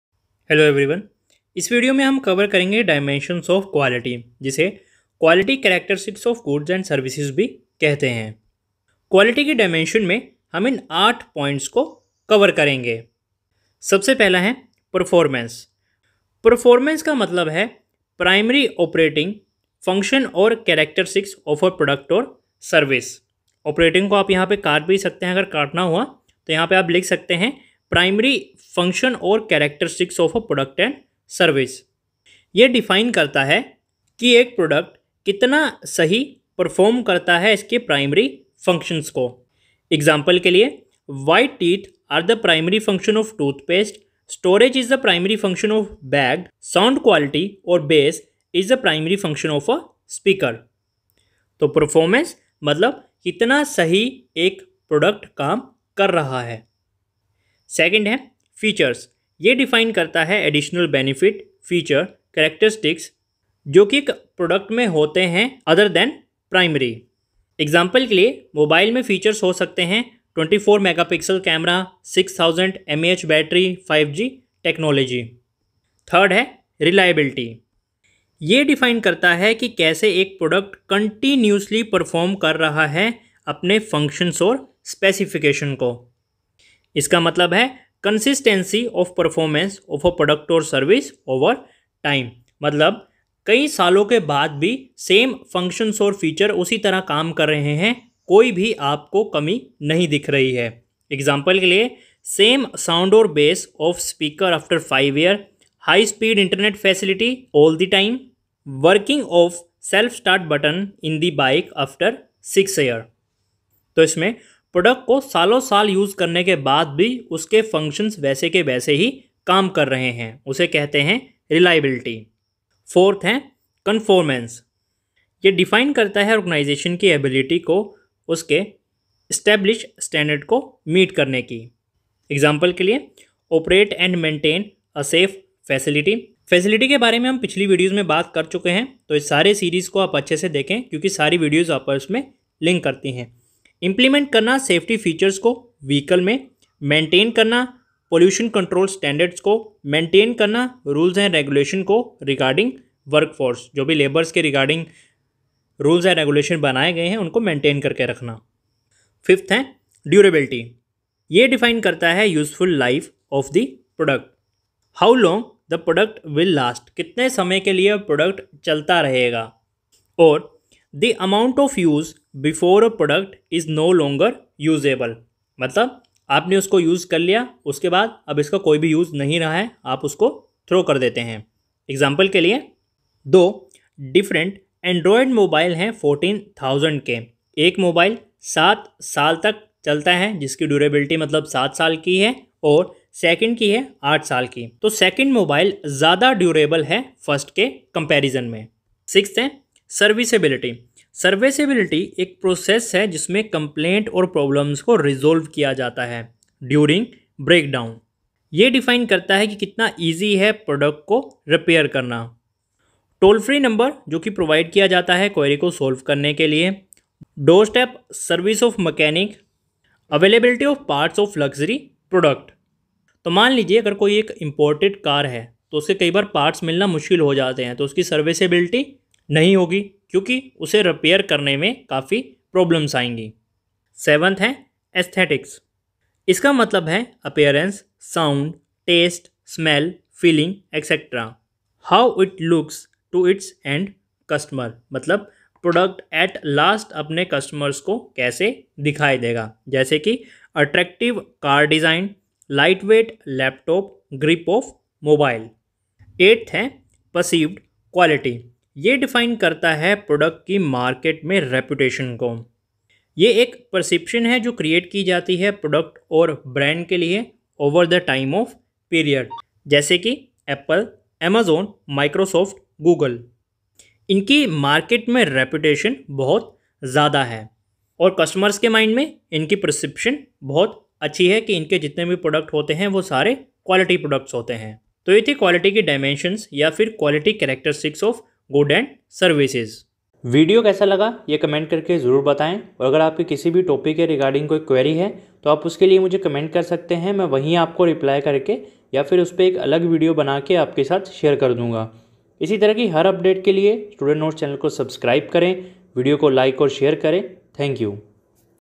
No.54 ~ Dimension of Quality or Quality Characteristics of Good and Services
📕 Production and Operation Management video Series 📕

📕 Advertising Management Book series 📕

✅ How to Purchase Production and Operation Management Book PDF ✅

Dimensions of Quality

00:00 Starting
00:14 Performance
00:57 Features
00:16 Reliability
02:03 Conformance
02:39 Durability
03:19 Serviceability
03:54 Aesthetics
04:16 Perceived quality

1.) Performance

Performance defines the primary operating function or characteristics of a product or service. It defines that how well it performs its primary functions.

2.) Features

It defines the additional benefit, feature, characteristics that a product has other than the primary.

3.) Reliability

It defines that how a product will perform continuously with its function and specifications. It means the consistency of performance of products or service’s overtime.

4.) Conformance

It defines the ability of the organization to meets its established standard.

5.) Durability

It defines the useful life of the product. How long the product will last or the amount of use before a product is no longer usable.

6.) Serviceability

Serviceability is the process of resolve the complaint and problems during the breakdown. It defines that how easy it is to repair the product.

7.) Aesthetics

It means appearance, sound, taste, smell, feeling, etc. How it looks to its end consumer.

8.) Perceived quality

It defines the reputation of the product in the market. It is the perception created for the products or brand over the period of time.